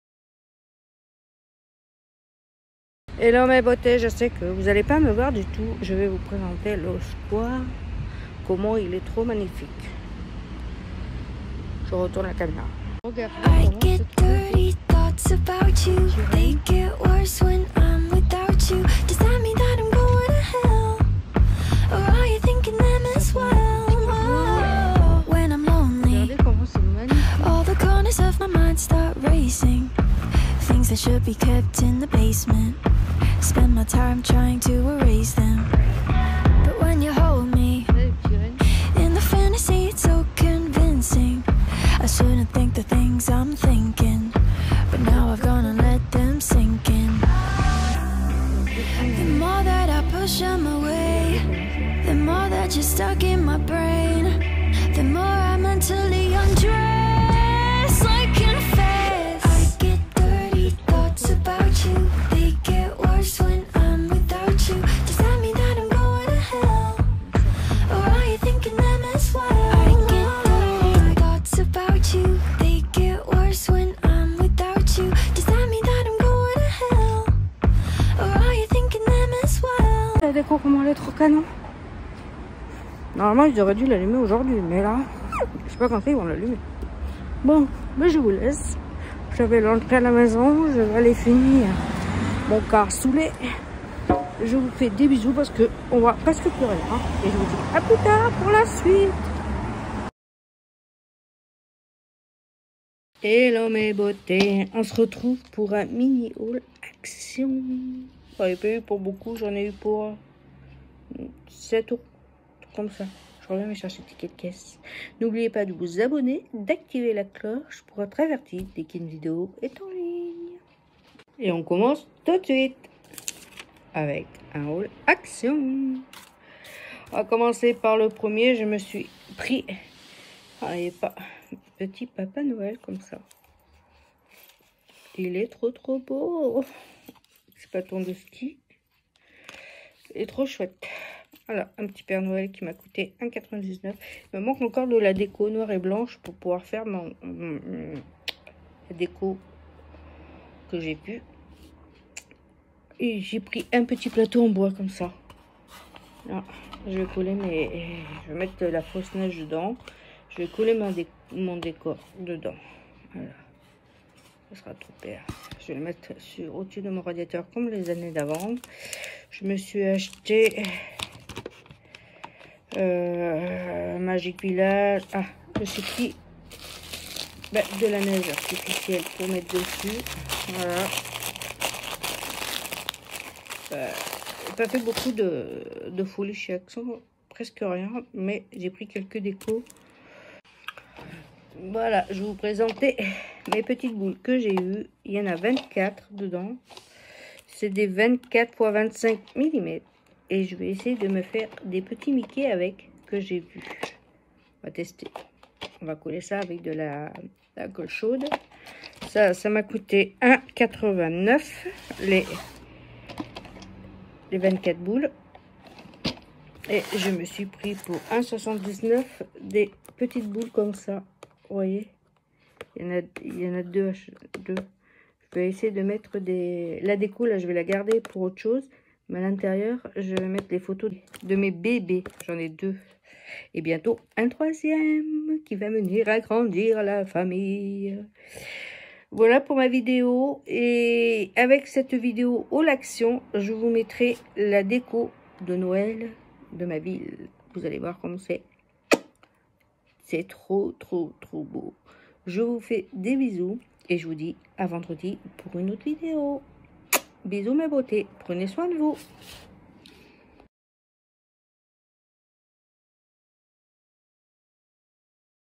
Deco de ma ville et haul action
Hello mais voter on se retrouve pour une vidéo je vous présente tous les Décos de ma ville qui sont trop trop trop trop canon j’espère que vous allez apprécier ces vidéos que je vous tourne un petit peu tous les jours.

Et on se retrouve pour un haul action

J’espère que mes vidéos vous plaît je vous fais un bisou et je vous dis à plus tard￼

Mon uTip

♥️bisous ♥️